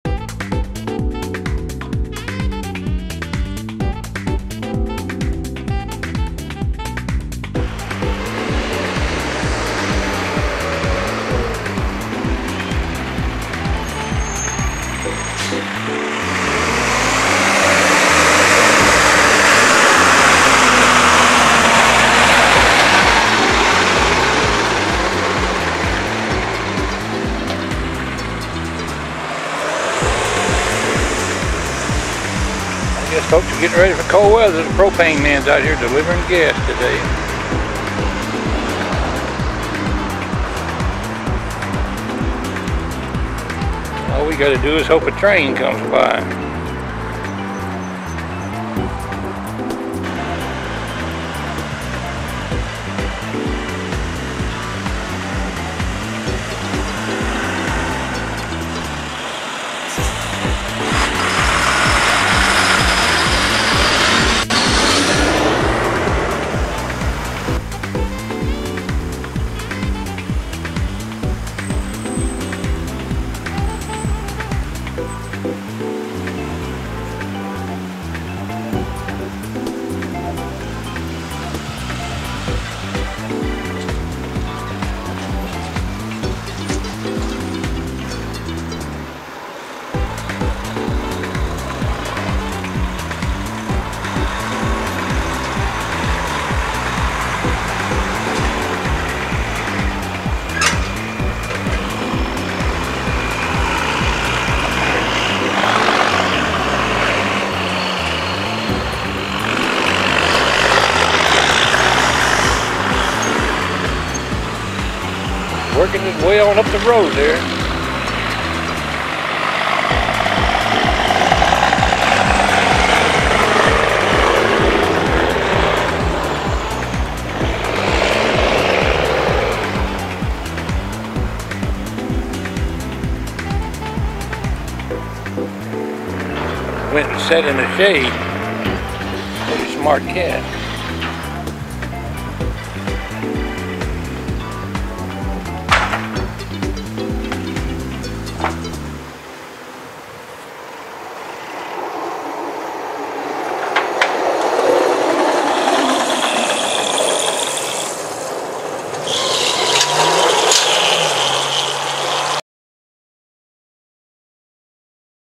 Propane Trucks & A Cat.mpg
Just a bit of extrarailfan footage,  I didnt want to waste.  Video of A Cat in Jackson GA. that seemed to have a 6th sense or ESP or something, and A couple of propane trucks at Jenkinsburg Georgia.